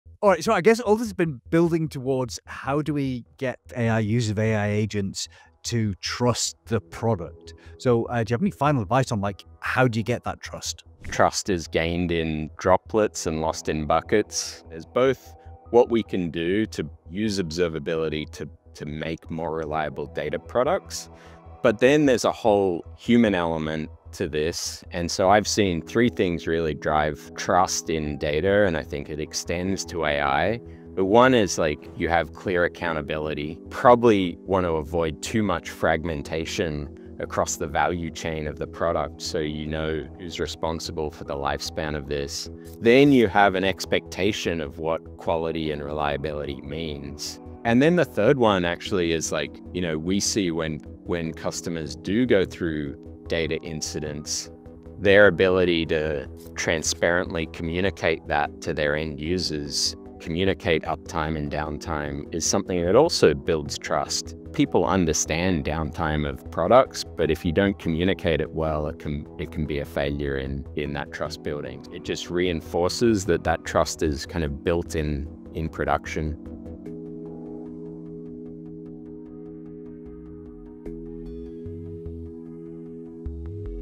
Hot to Build Trust in your AI products?
Data quality and AI reliability are two sides of the same coin in today's technology landscape. Organizations rushing to implement AI solutions often discover that their underlying data infrastructure isn't prepared for these new demands. But what specific data quality controls are needed to support successful AI implementations? How do you monitor unstructured data that feeds into your AI systems? When hallucinations occur, is it really the model at fault, or is your data the true culprit? Understanding the relationship between data quality and AI performance is becoming essential knowledge for professionals looking to build trustworthy AI systems.

Shane Murray is a seasoned data and analytics executive with extensive experience leading digital transformation and data strategy across global media and technology organizations. He currently serves as Senior Vice President of Digital Platform Analytics at Versant Media, where he oversees the development and optimization of analytics capabilities that drive audience engagement and business growth. In addition to his corporate leadership role, he is a founding member of InvestInData, an angel investor collective of data leaders supporting early-stage startups advancing innovation in data and AI. Prior to joining Versant Media, Shane spent over three years at Monte Carlo, where he helped shape AI product strategy and customer success initiatives as Field CTO.

Earlier, he spent nearly a decade at The New York Times, culminating as SVP of Data & Insights, where he was instrumental in scaling the company’s data platforms and analytics functions during its digital transformation. His earlier career includes senior analytics roles at Accenture Interactive, Memetrics, and Woolcott Research. Based in New York, Shane continues to be an active voice in the data community, blending strategic vision with deep technical expertise to advance the role of data in modern business.

In the episode, Richie and Shane explore AI disasters and success stories, the concept of being AI-ready, essential roles and skills for AI projects, data quality's impact on AI, and much more.

Related Episode: Scaling Data Quality in the Age of Generative AI with Barr Moses, CEO of Monte Carlo Data, Prukalpa Sankar, Cofounder at Atlan, and George Fraser, CEO at Fivetran

New to DataCamp?